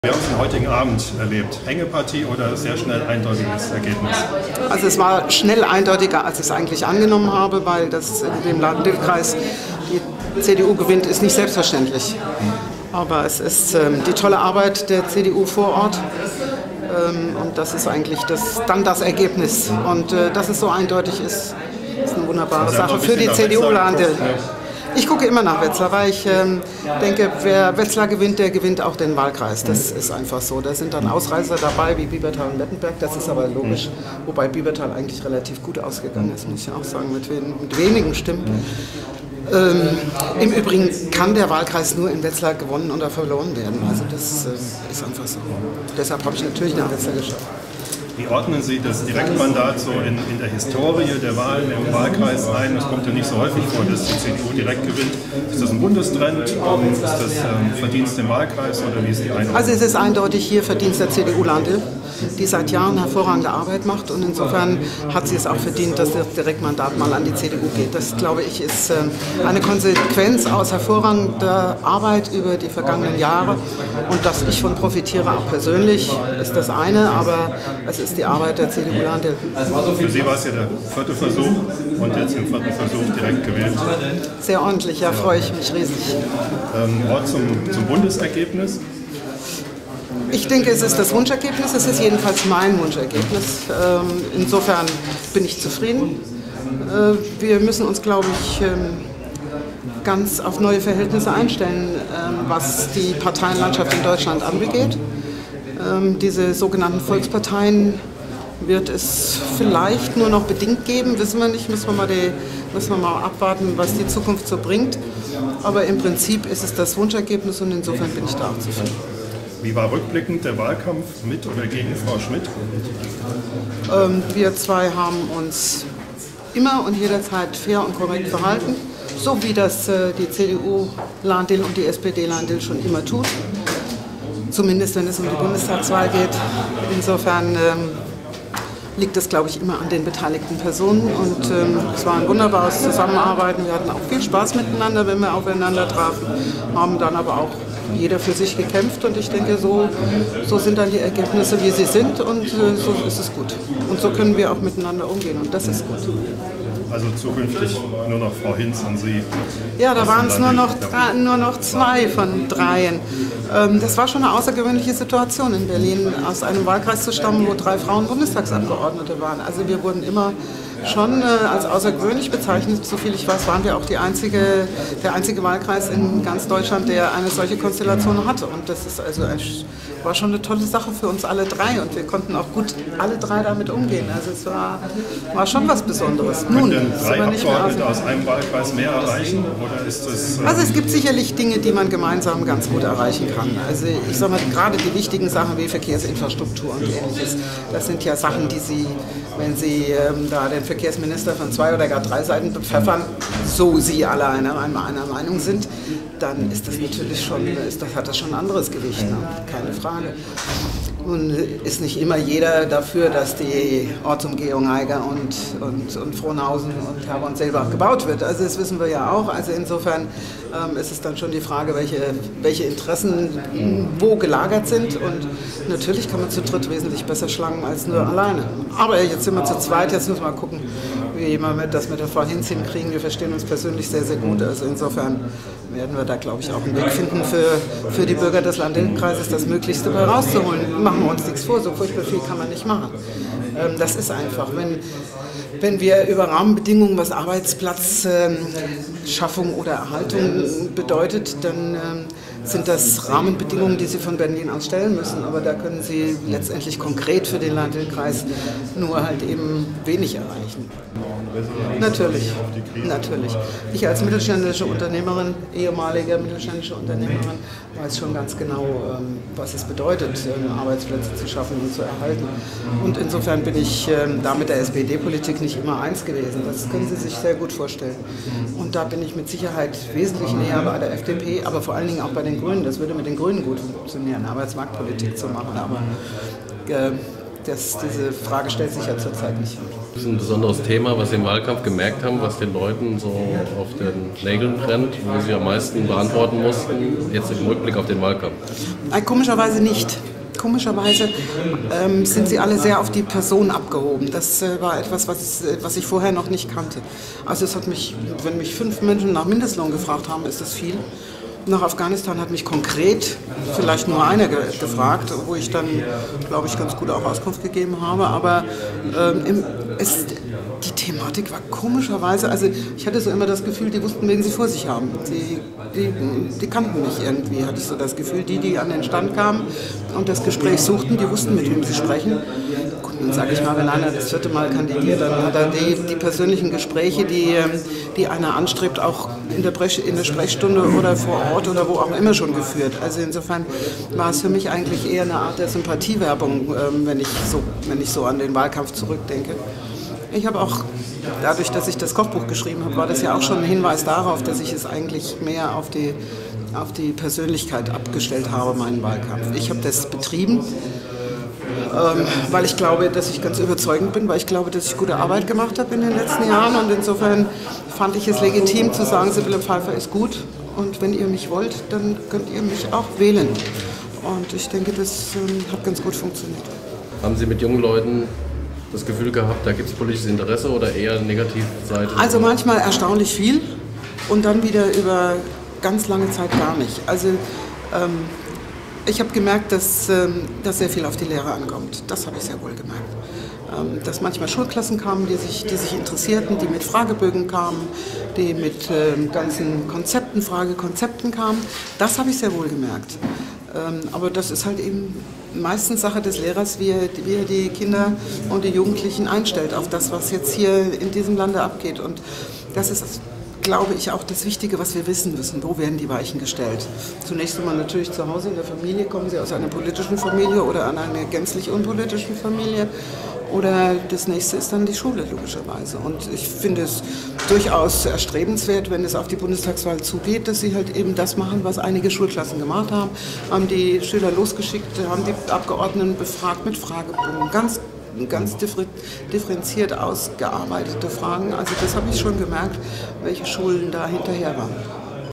Nach der Wahl: Sibylle Pfeiffer (MdB/CDU)
Bundestagsabgeordnete Sibylle Pfeiffer (MdB/CDU)  nimmt Stellung zum Wahlerfolg der CDU und zu ihrem Gewinn des Direktmanadats.